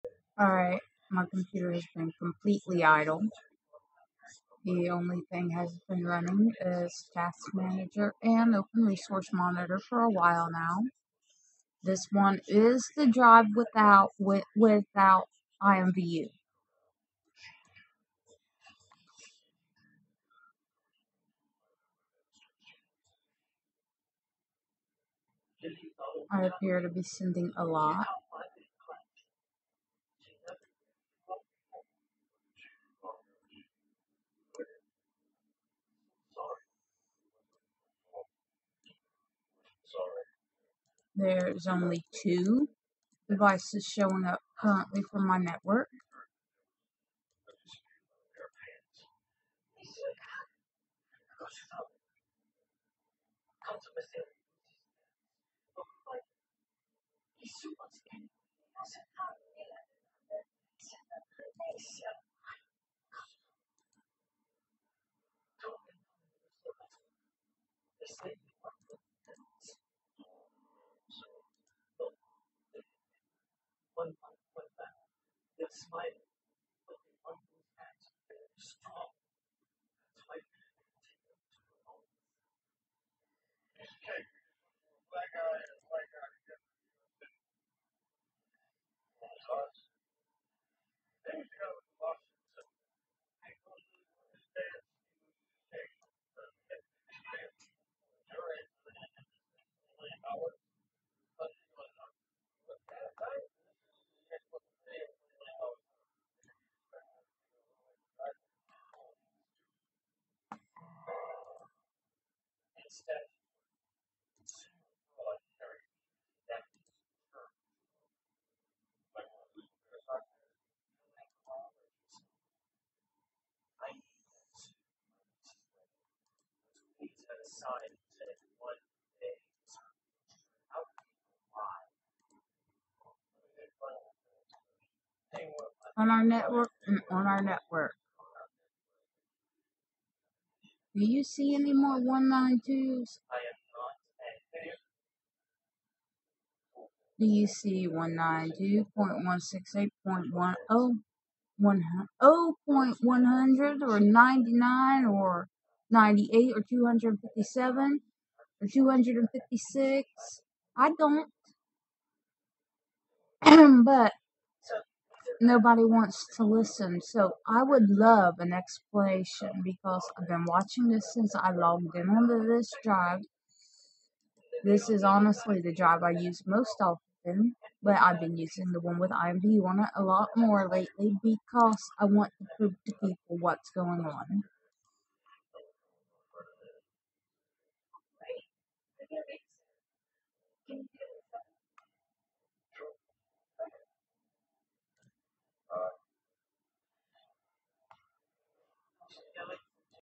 still not there.. so I'm waiting for the explanation no one has
Why is it with 2 drives with nearly identical setups. Only one seems to have a million connections that seem to be on my network? ONLY on drive? And the major difference being 2 programs... 2 that's it! And considering this has been happening before I installed TCPView, I doubt it is what is causing this? Even though I had not only my network name changed but the password as well. I have a ton more activity than I should, but only on ONE drive?!?

After documenting their illegal acts and contacting their CEO to inform them that if they are the ones behind it, it is a breach of their TOS, I no longer see activity from IO CAPITAL PRINCESS (data collection company) Instead, I have all of these addresses appearing to be on our home network, popping up one after another. All connections I'm sending to but not receiving from?!?

IS IMVU STEALING FROM YOU TOO?